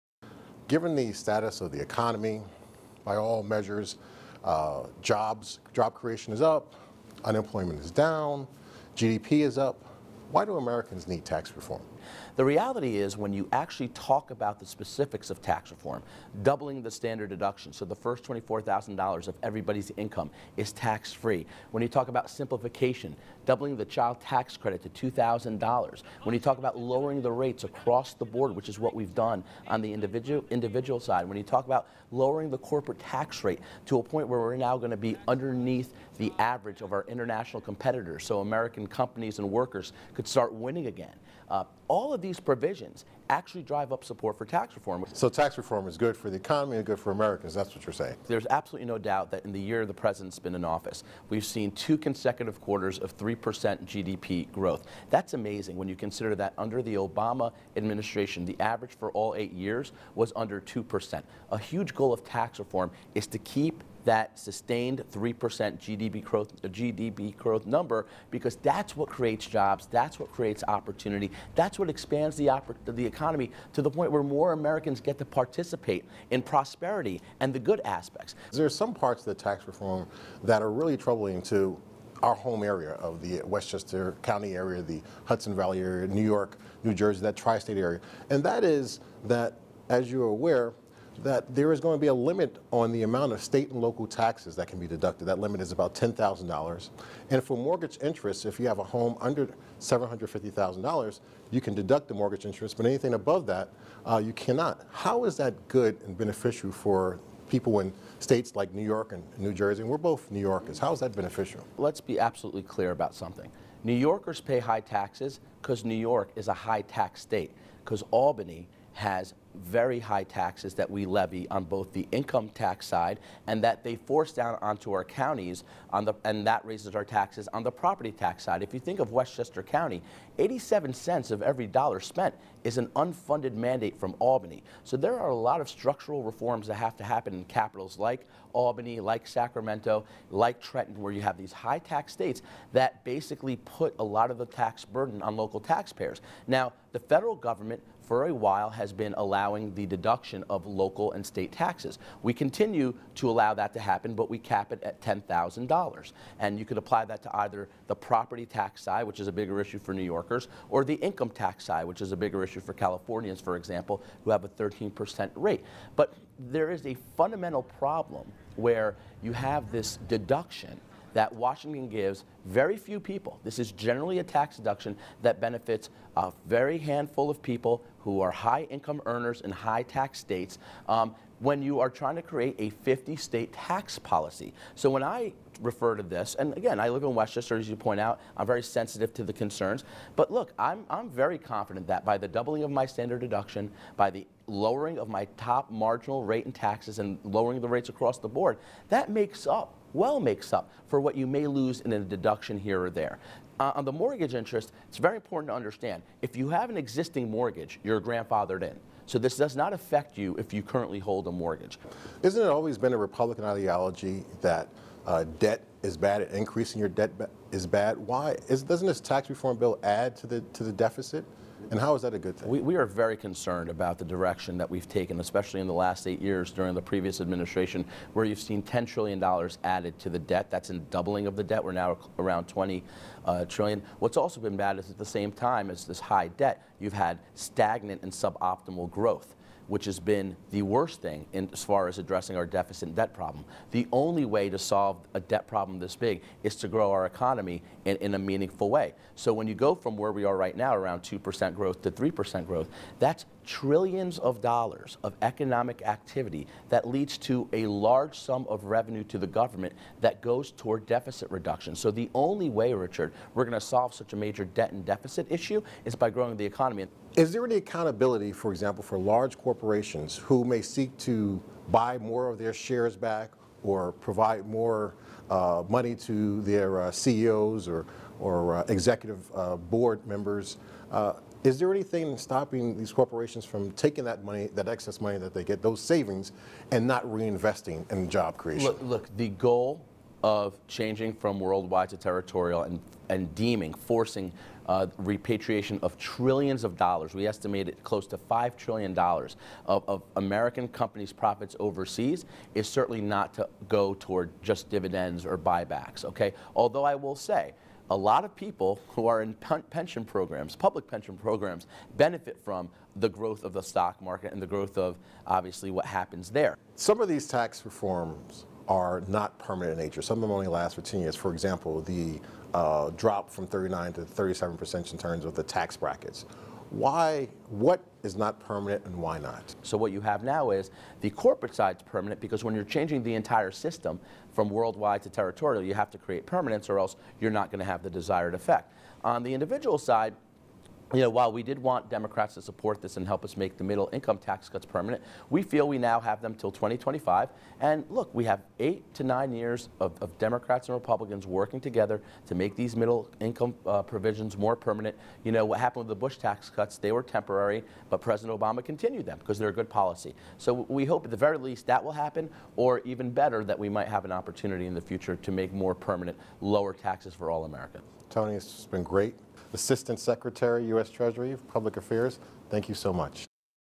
Richard St. Paul, Esq. talks with Tony Sayegh
Richard St. Paul Esq. talks with Tony Sayegh, Assistant Secretary for Public Affairs for the U.S. Department of the Treasury.